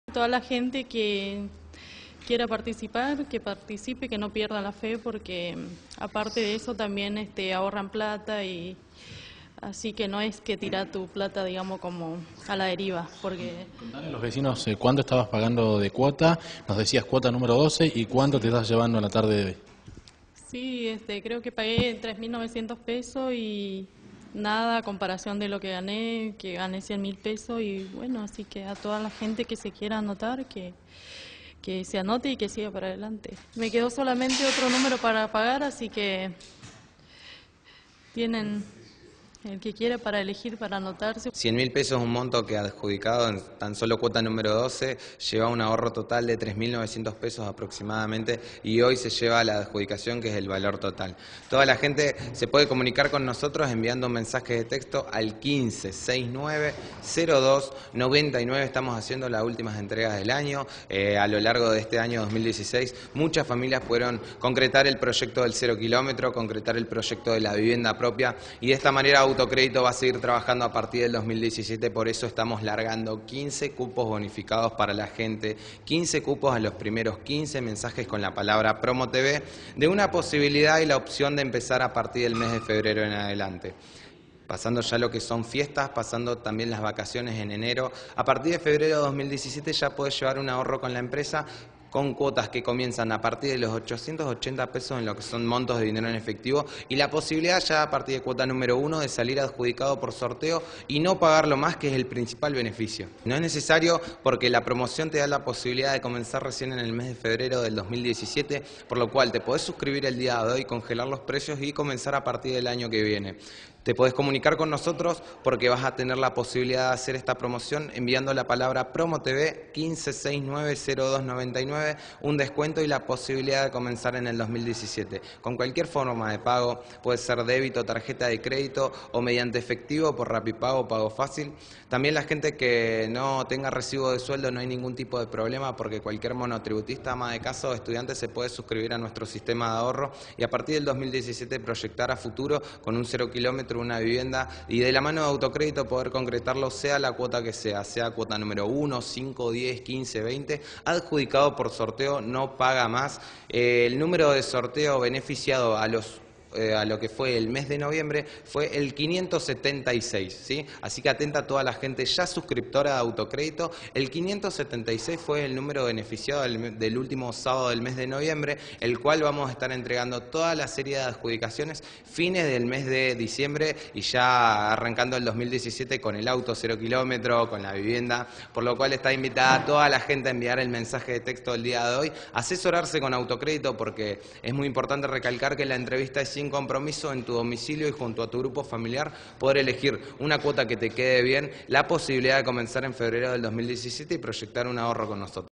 GERARDO KAMMAN- AUTOCRÉDITO - ENVIÁ “PROMO TV” AL 02966 690299 Y SUMATE
SOFÍA ES UNA NUEVA ADJUDICATARIA DE LA EMPRESA AUTOCRÉDITO.
DIALOGAMOS CON ELLA QUIEN ADJUDICÓ LA SUMA DE 100 MIL PESOS Y CON GERARDO KAMMAN QUIEN SE REFIRIÓ A LAS GRANDES VENTAJAS DE SUMARSE A LA FAMILIA AUTOCRÉDITO.